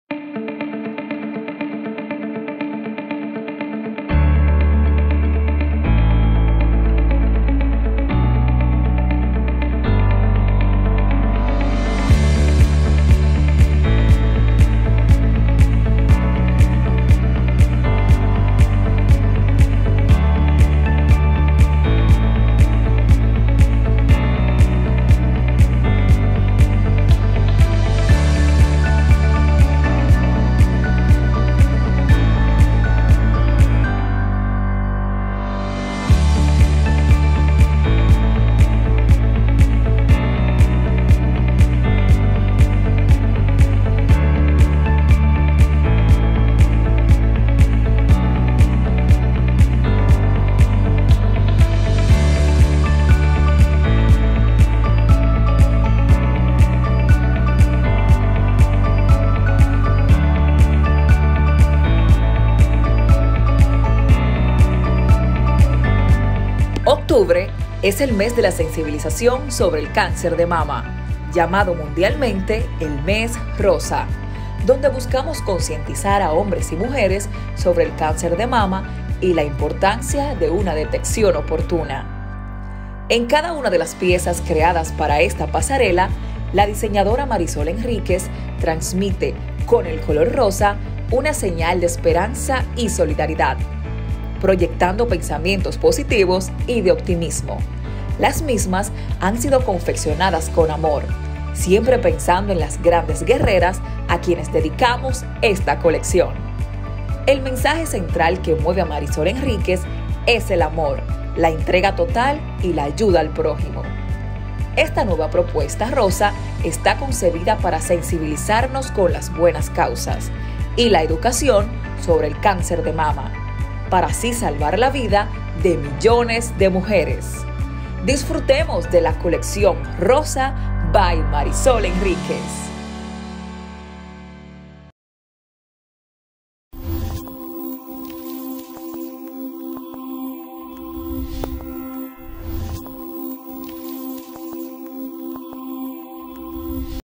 Colección “Rosa by Marysunhenriquez Octubre es el Mes de Sensibilización sobre el Cáncer de Mama HD
Colección “Rosa by Marysunhenriquez Octubre es el Mes de Sensibilización sobre el Cáncer de Mama, llamado mundialmente el MES ROSA, donde buscamos concientizar a hombres y mujeres sobre el cáncer de mama y la importancia de una detección oportuna.
En cada una de las piezas creadas para esta pasarela, la diseñadora Marisol Henríquez trasmite, con el color rosa, una señal de esperanza y solidaridad, proyectando pensamientos positivos y optimismo.
El rosa es un color emocional que influye en nuestros sentimientos convirtiéndolos en amables, suaves y profundos, y todas las piezas de esta colección evocan afecto, amor y protección.
Si nos remontamos a las esencia del rosa, color derivado del rojo mitigado por la pureza del blanco, podemos sentir la influencia y fortaleza del rojo que refleja la parte más sexual de las personas que se une en una danza con el blanco para sentir el amor altruista y verdadero, sumando romanticismo, encanto, belleza, dulzura, delicadeza, refinamiento, calma y ternura.
Inocencia, amor, entrega total y ayudar al prójimo es el mensaje central que mueve a Marisol Henríquez a dar vida a esta nueva propuesta rosa para sensibilizarlos con las buenas causas a favor de la educación y concienciación de la detección temprana del Cáncer de mama. Unamos en esta campaña que puede salvar la vida de millones de mujeres. “Colección Rosa by Marysunhenriquez  @hanovidangelic @popfashionweek